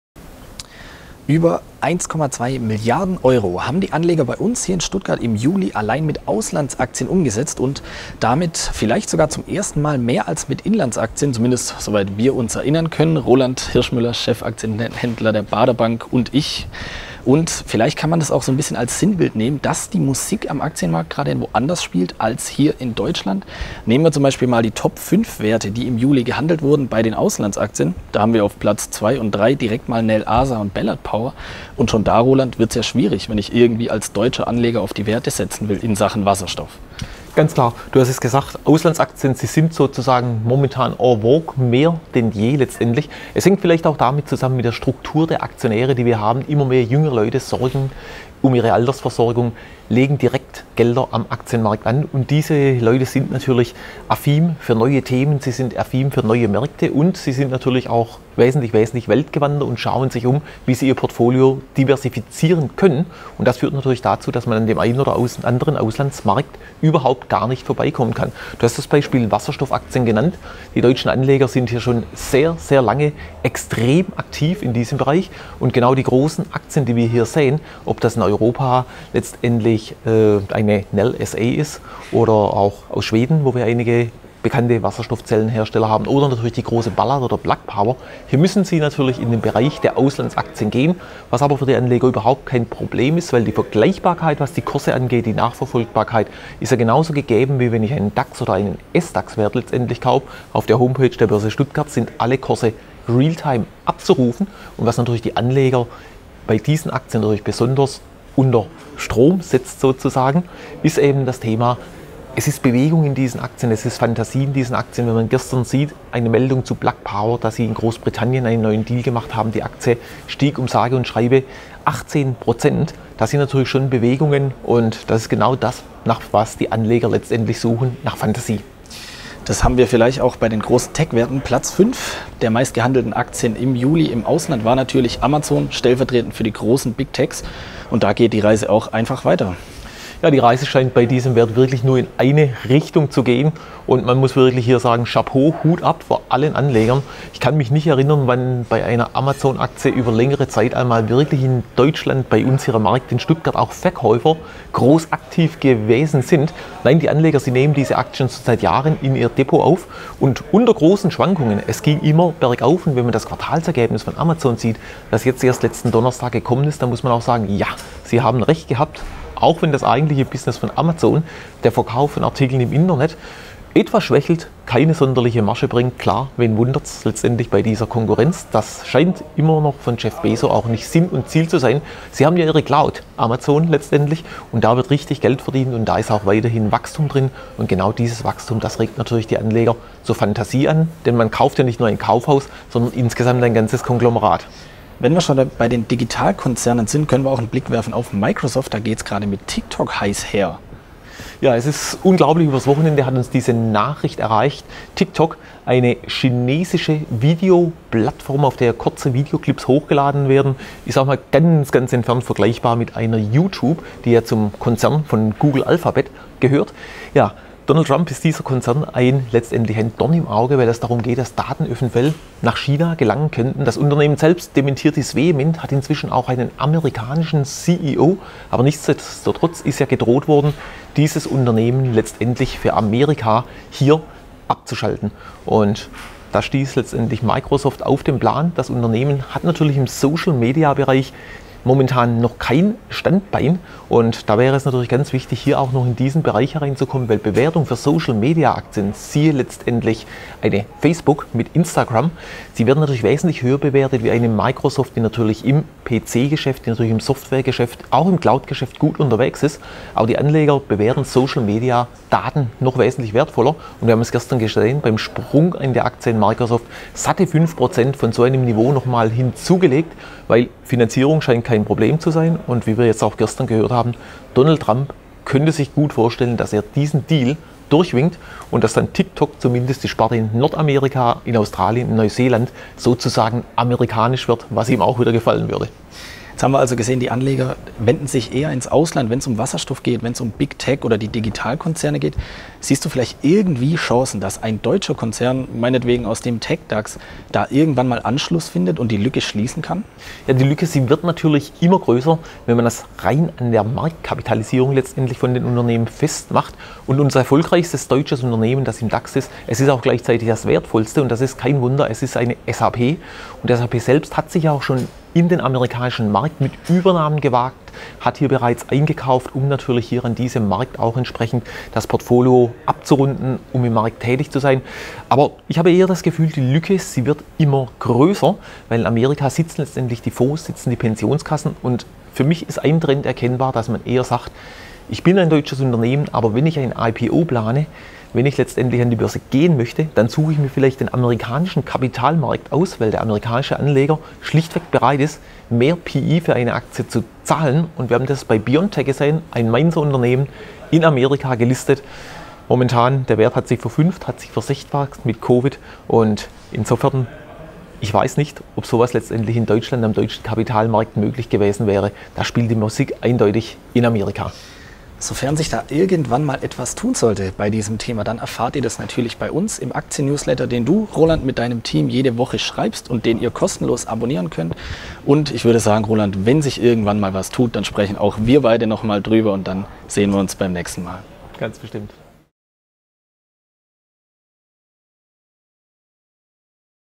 Wasserstoff & Tech-Aktien: Darum handeln Anleger immer mehr Auslandswerte | Börse Stuttgart
► Mit einem Volumen von mehr als 1,2 Milliarden Euro handelten die Stuttgarter Anleger im Juli mehr Auslandsaktien als heimische Werte. Warum die Musik derzeit nicht auf dem deutschen Markt spielt und was Microsoft mit dem TikTok-Deal bezwecken will, berichtet Roland Hirschmüller, Auslandsexperte der Börse Stuttgart, im Interview.

00:00 ► Darum werden mehr Auslandsaktien gehandelt
02:09 ► Tech-Aktien hängen den deutschen Markt ab
03:15 ► Microsoft & der TikTok-Deal
04:46 ► Der Trend geht weg von Deutschland